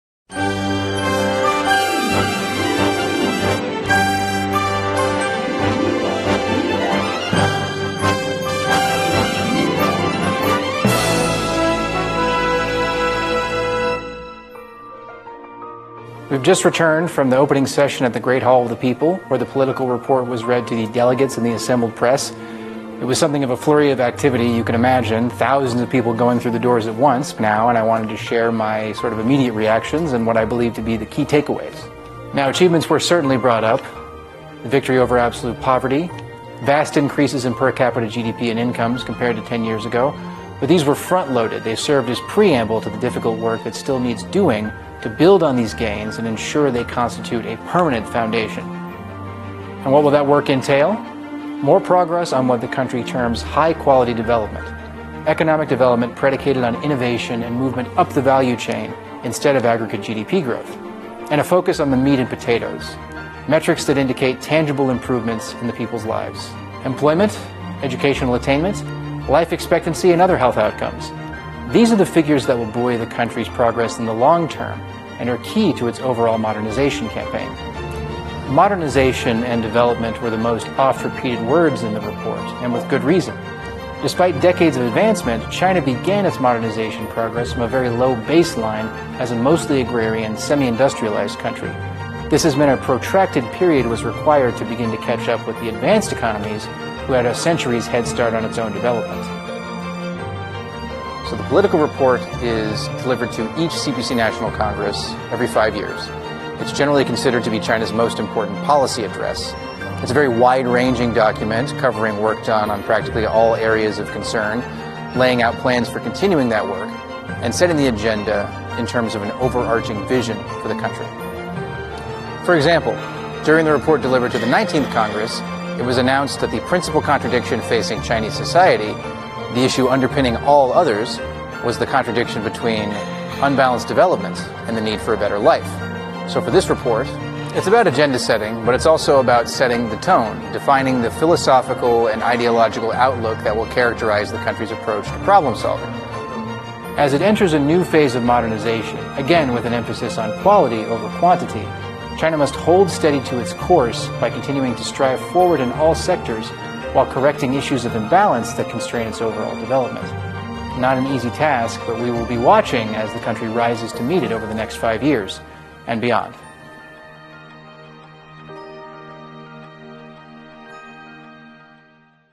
20thcpcnationalcongress ——Key takeaway from the report (美国共产党员来到二十大现场)
Communist Party USA came to the scene of the 20thcpcnationalcongress . (美国共产党·我来到了二十大现场)